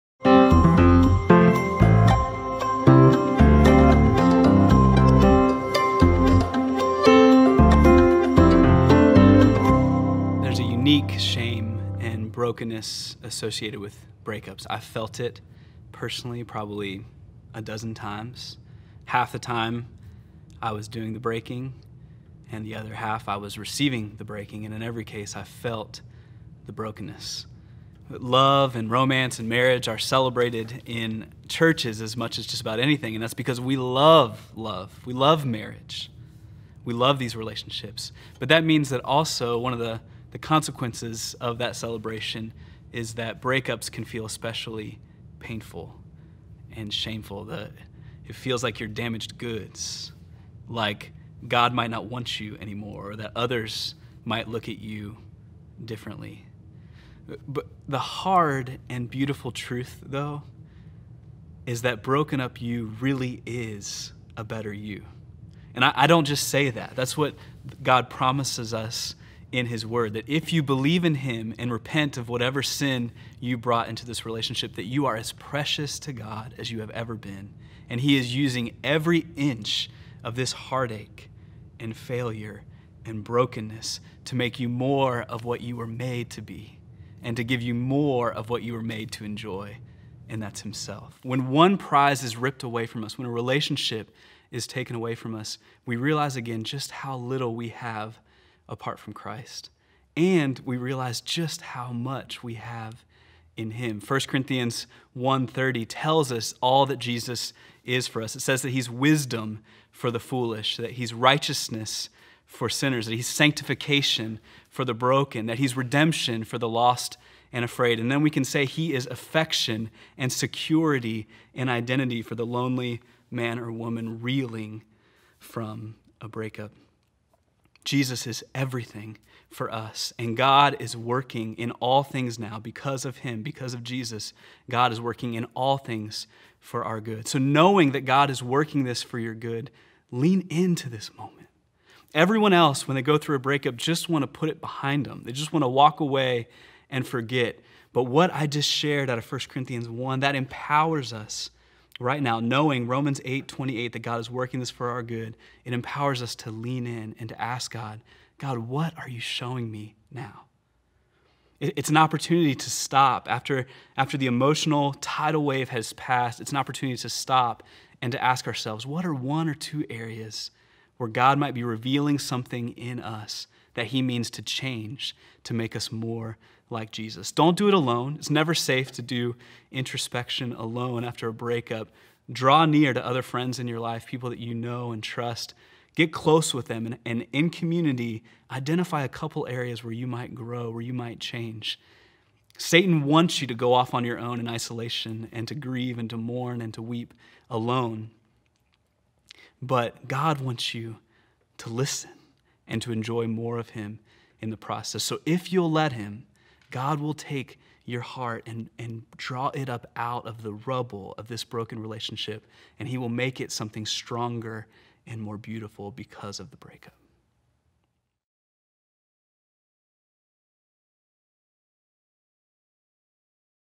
You Are Better Having Loved and Lost
There’s a unique shame and brokenness associated with breakups. Relationships and love may be celebrated more in the church than anywhere else because we (rightly) love marriage so much. Unfortunately, these same convictions often make breakups an uncomfortable conversation — at best embarrassing and at worst scandalous or humiliating.

You feel like damaged goods, like you’ve been ruined in God’s eyes or in the eyes of others. The hard-to-believe, but beautiful truth is that broken-up you is a better you. If in your sorrow you turn to the Lord and repent of whatever sin you brought to this relationship, you are as precious to your heavenly Father as you have ever been, and he is using every inch of your heartache, failure, or regret to make you more of what he created you to be and to give you more of what he created you to enjoy — himself.